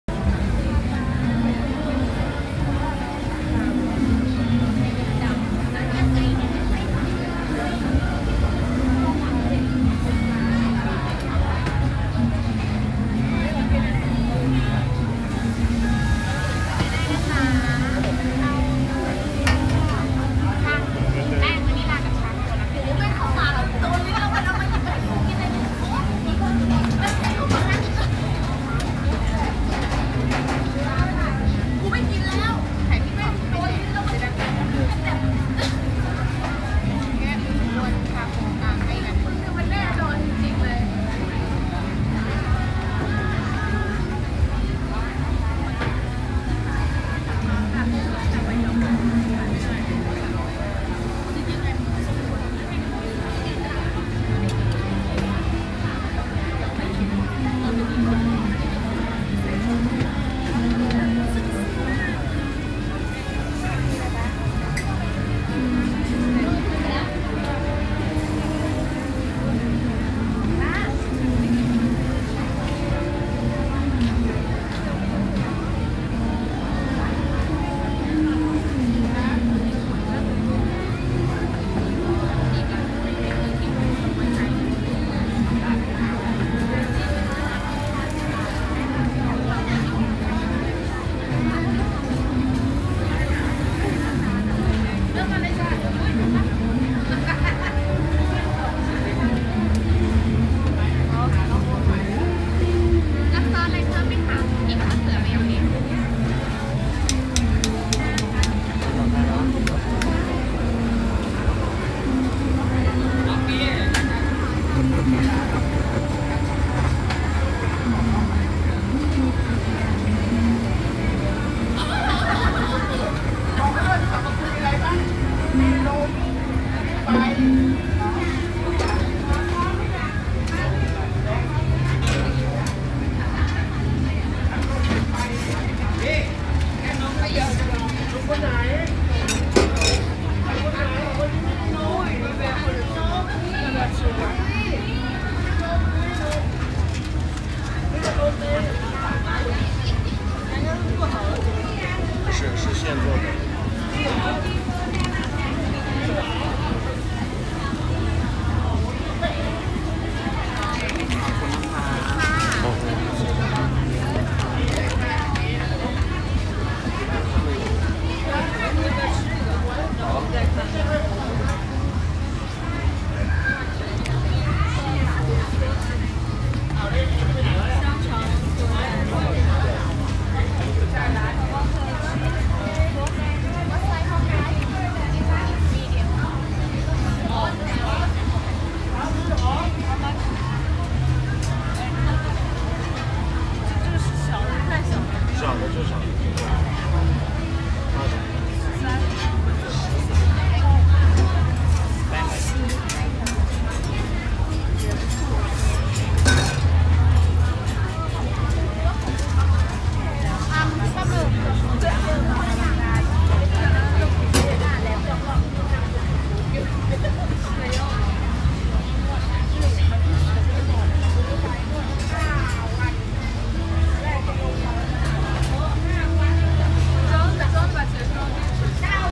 CRISPY BLACK CREPE PIZZA - THAI CREPE DESSERT | Bangkok Street Food, Thailand
This Bangkok street food, is a crispy black crepe pizza, a Thai crepe dessert.

The paste is charcoal flavor, which gives it that black color. Hence the term black crepe.

You choose the type of topping you want and they will make it fresh on the spot for you. So it's still quite hot.

Price: 39thb (price varies according to size and toppings).
Location: Srinakarin Night Market

★ CONNECT WITH ONE LIFE ON SOCIAL MEDIA! ★

GEAR I USE:
Memory Card
External Hard drive